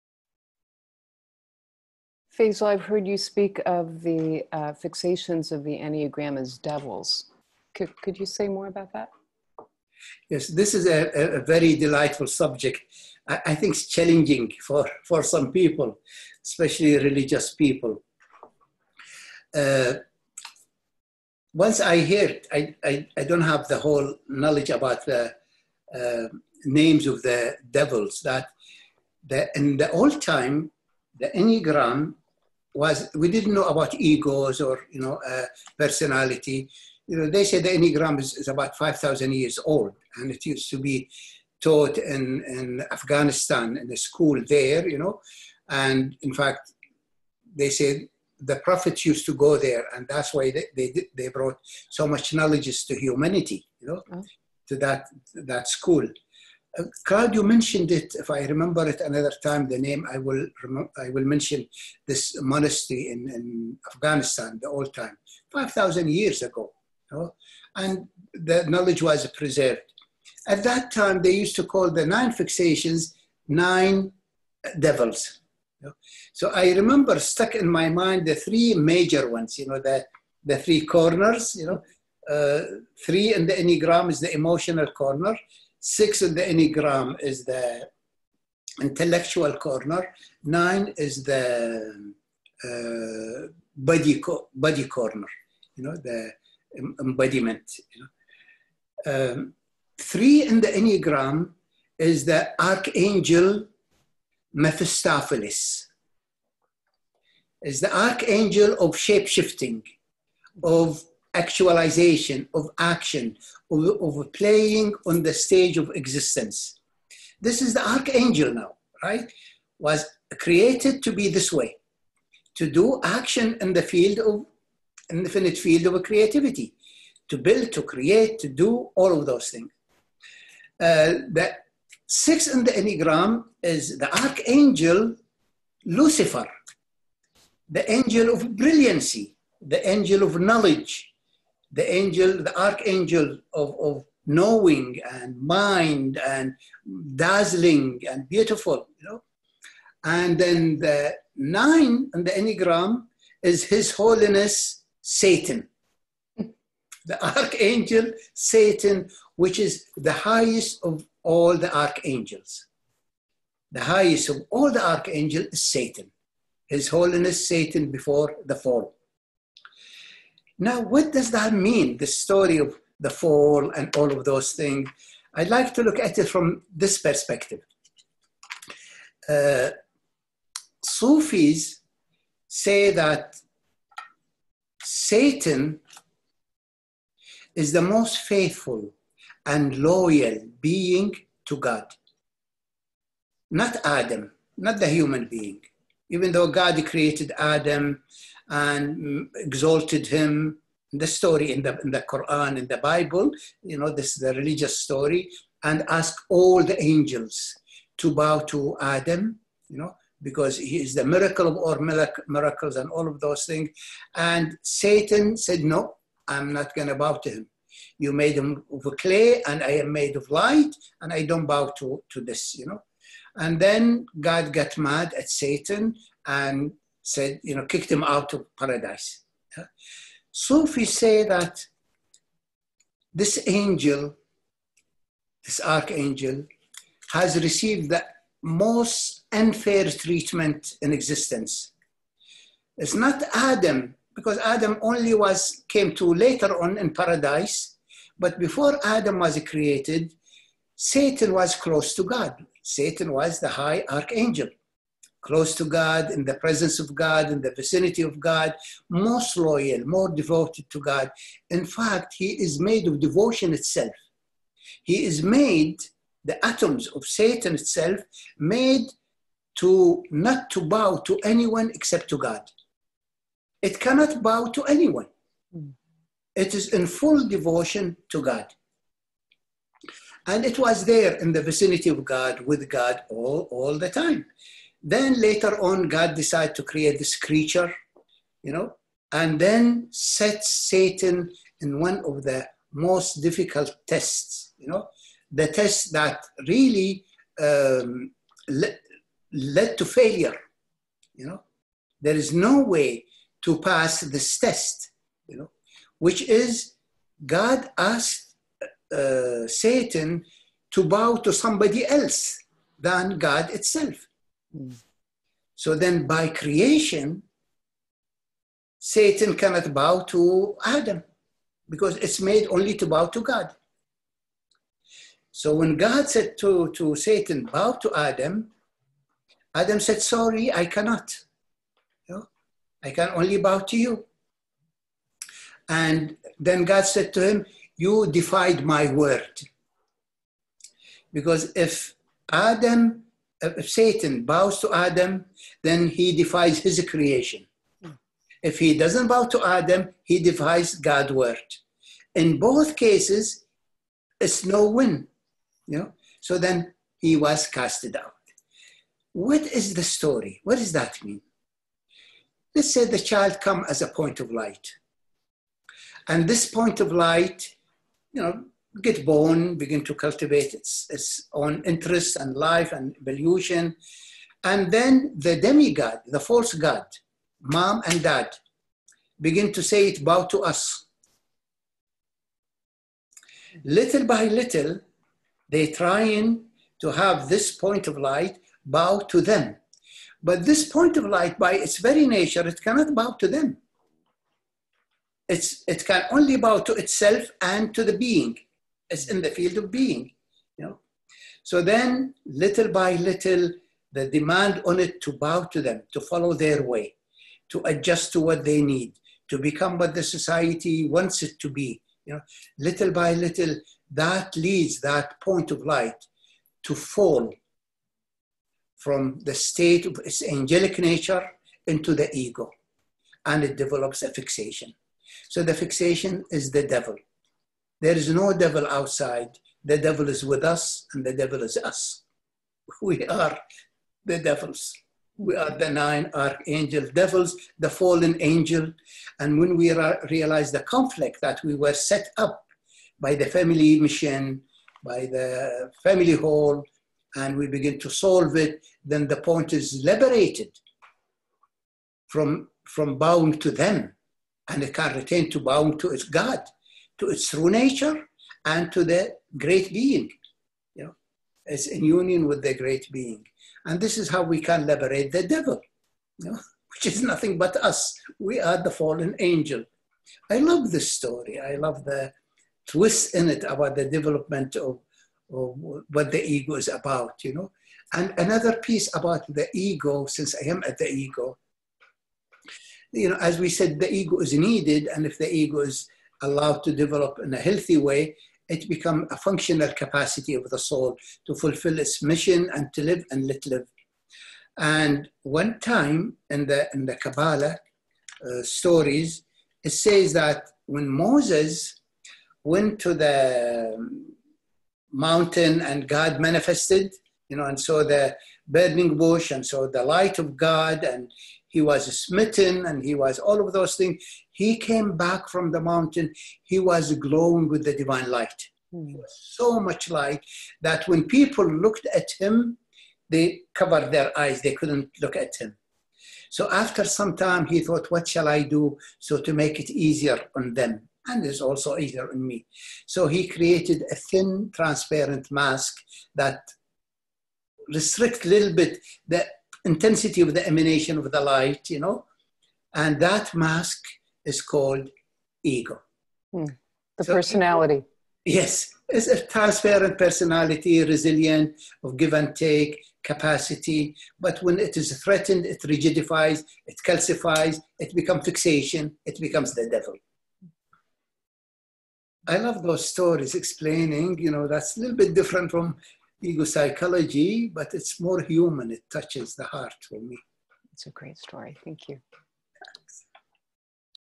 Devils and the Enneagram
In this interview, Faisal transfers the parable of Satan's expulsion from heaven to the enne-agram and thus to us. According to ancient sources the Enneagram originated in an Afghan monastery. It has nine fixations, which at that time were called the nine devils. The fixation nine stands for the highest archangel Satan. Consisting of pure devotion, his nature was to serve God exclusively. When God required Satan to bow to Adam contrary to his nature, the archangel refused and was banished. The parable represents our situation: we are fallen angels and have an essential self, the Point of Light. However, due to our conditioning, we had to turn away from our Point of Light. From now on we let our ego determine us.

In diesem Interview überträgt Faisal das Gleichnis von der Vertreibung Satans aus dem Himmel auf das Enneagramm und damit auf uns. Das Enneagramm stammt alten Quellen zufolge aus einem afghanischen Kloster. Es weist neun Fixierungen auf, die damals als die neun Teufeln bezeichnet wurden. Die Fixierung Neun steht für den höchsten Erzengel Satan. Bestehend aus reiner Hingabe entsprach es seinem Wesen, ausschließlich Gott zu dienen. Als Gott von Satan verlangte, sich entgegen seiner Natur vor Adam zu verbeugen, weigerte sich der Erzengel und wurde verbannt. Das Gleichnis steht für unsere Situation: Wir sind gefallene Engel und haben ein essenzielles Selbst, den Point of Light. Aufgrund unserer Konditionierung mussten wir uns jedoch von unserem Point of Light abwenden. Fortan lassen wir uns von unserem Ego bestimmen.